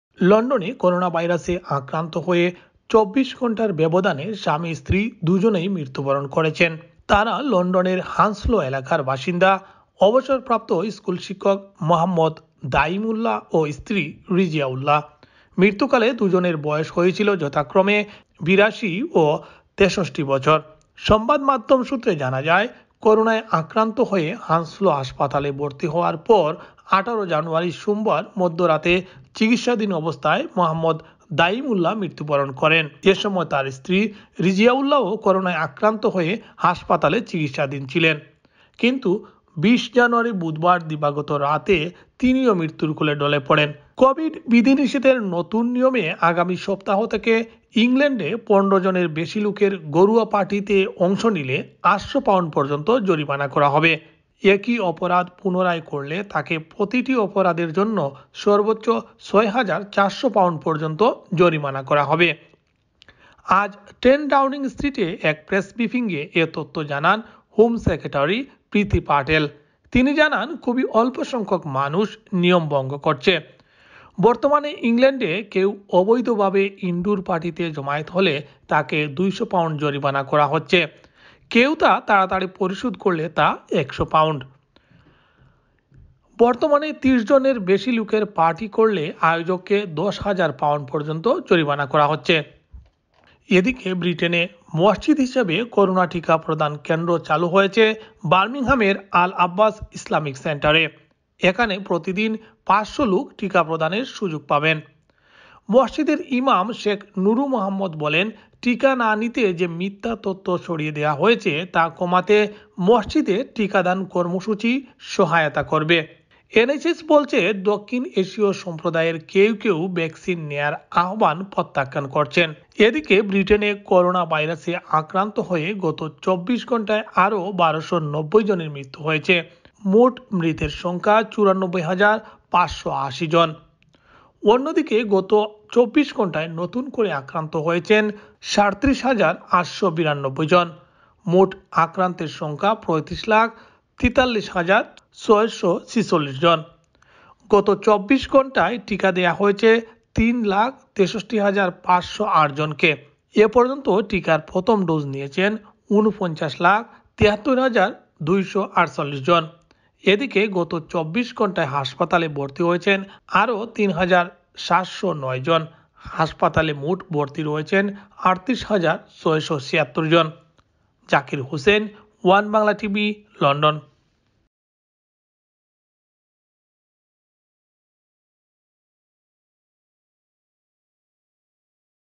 লন্ডনে করোনায় স্বামী-স্ত্রী মৃত্যু, ঘরোয়া পার্টিতে অংশ নিলে ৮০০পাউন্ড জরিমানা,করোনায় ১২৯০জনের মৃত্যু
ব্রিটেনে করোনায় একদিনের ব্যবধানে স্বামী-স্ত্রী মৃতু: ঘরোয়া পার্টিতে অংশ নিলে ৮০০ পাউন্ড জরিমানা, করোনায় ১২৯০ জনের মৃত্যু
ব্রিটেনে প্রথম মসজিদ হিসেবে করোনা টিকা প্রদানের কেন্দ্র চালু হয়েছে বামিংহামের আল আব্বাস ইসলামিক সেন্টারে